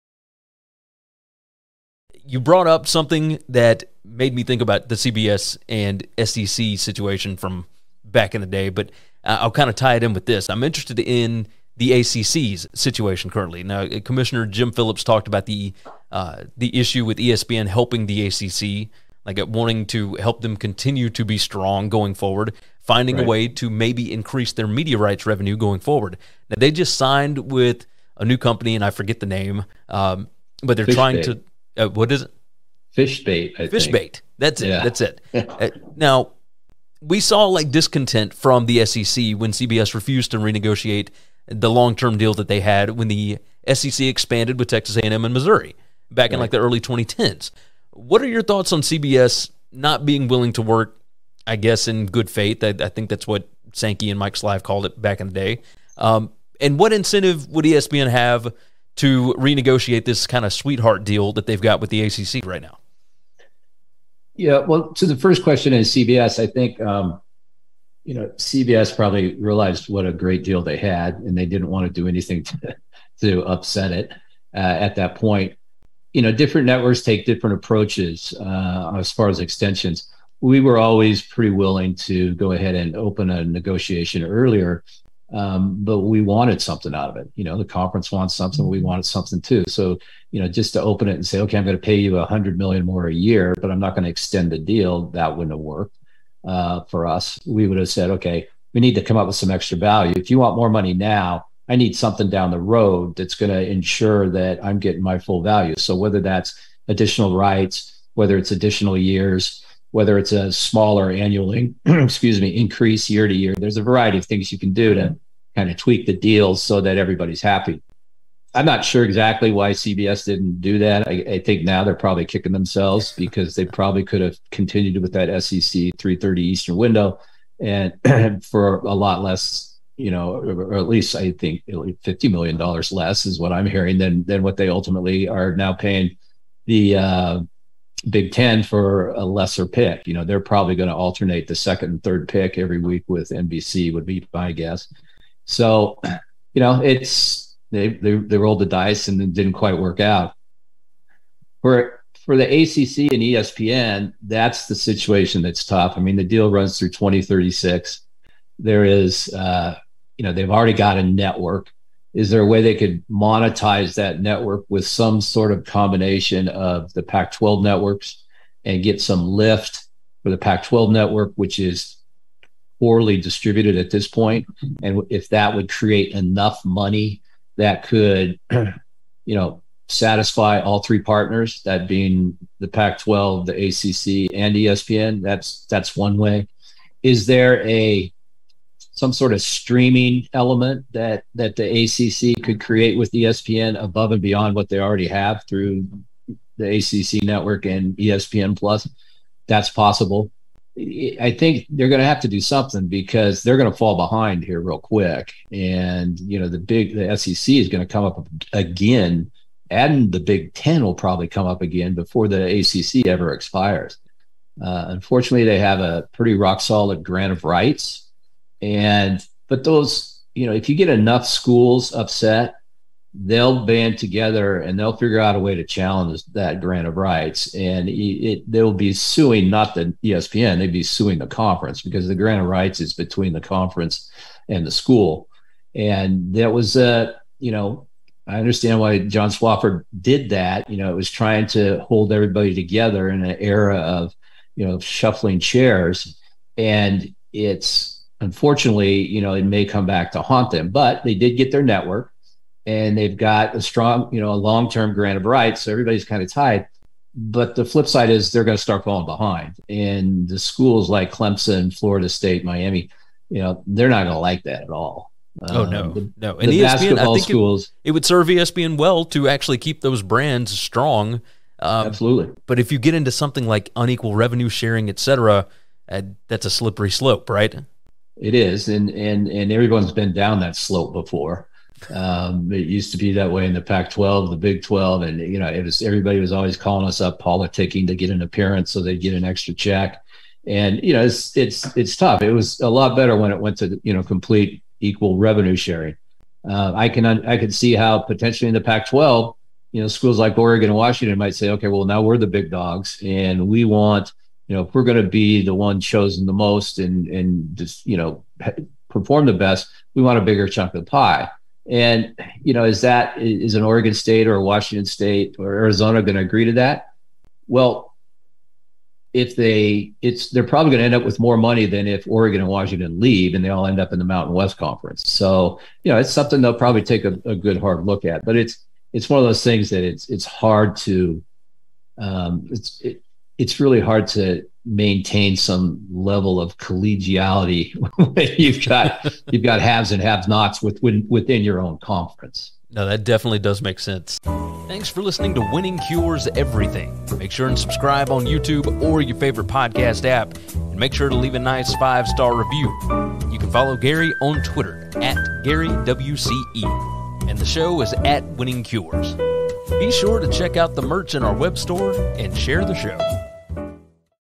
ESPN renegotiate ACC media rights? Is there any incentive for a new TV deal?
The ACC has stated that ESPN wants to help them maintain their strength through this round of realignment.  Their tv deal & grant of rights doesn't expire until 2036.  Does ESPN have any incentive to renegotiate the current ACC media rights deal?

Bob Thompson, former Fox Sports President, (@RLTSports on Twitter) joined the show to talk about media rights for the Big Ten, ACC, Big 12, Pac 12, College Football Playoff Expansion, value of specific CFB time slots, Notre Dame, realignment and expansion, and more!

Auto Transcription:
 I'm interested in the ACC's
 Situation currently now Commissioner Jim Phillips talked about the  the issue with ESPN helping the A. C. C.
 Like wanting to help them continue to be strong going forward.
 Finding a way to maybe increase their media rights revenue going forward now.
 They just signed with a new company and I forget the name.
  But they're trying to, what is fish bait That's it, that's it. Now.
 We saw like discontent from the sec when CBS refused to renegotiate the long term deal that they had when the sec expanded with Texas A&M in Missouri back in like the early 2010s.
 What are your thoughts on Cbs not being willing to work I guess in good faith that I think that's what spanky and mike's life called it back in the day.
  And what incentive would ESPN have to renegotiate this kind of sweetheart deal that they've got with the ACC.
 Right now. Yeah. Well to the first question is CBS I think  you know CBS probably realized what a great deal they had and they didn't want to do anything to upset it at that point.
 You know different networks take different approaches  as far as extensions we were always pretty willing to go ahead and open a negotiation earlier.
  But we wanted something out of it. You know the conference wants something we wanted something too.
 So you know, just to open it and say okay I'm gonna pay you 100 million more a year.
 But I'm not gonna extend the deal that wouldn't have worked  for us.
 We would have said okay we need to come up with some extra value.
 If you want more money now, I need something down the road that's gonna ensure that I'm getting my full value.
 So whether that's additional rights, whether it's additional years, whether it's a smaller annually, excuse me, increase year to year.
 There's a variety of things you can do to kind of tweak the deal so that everybody's happy.
 I'm not sure exactly why Cbs didn't do that.
 I think now they're probably kicking themselves because they probably could have continued with that sec.
 3 30 eastern window and for a lot less, you know, at least I think $50 million less is what I'm hearing than than what they ultimately are now paying the big 10 for a lesser pick.
 You know, they're probably gonna alternate the 2nd and 3rd pick every week with NBC would be my guess.
 So, you know, it's they rolled the dice and it didn't quite work out for for the A. C. C.
 And E. S. P. N. That's the situation. That's tough. I mean the deal runs through 2036.
 There is  you know, they've already got a network, is there a way they could monetize that network with some sort of combination of the pac 12 networks and get some lift for the pac 12 network which is poorly distributed at this point.
 And if that would create enough money that could you know, satisfy all three partners, that being the pac 12, the A. C. C.
 And E. S. P. N. That's that's one way is there a some sort of streaming element that that the A. C. C.
 Could create with the ESPN above and beyond what they already have through the A. C. C.
 Network and ESPN plus that's possible.
 I think they're gonna have to do something because they're gonna fall behind here real quick.
 And you know the big the sec is gonna come up again adding the Big 10 will probably come up again before the A. C. C.
 Ever expires. Unfortunately they have a pretty rock solid grant of rights and but those, you know, if you get enough schools upset, they'll band together and they'll figure out a way to challenge that grant of rights and it they will be suing nothing ESPN they'd be suing the conference because the grant of rights is between the conference and the school and that was you know, I understand why john Swofford did that.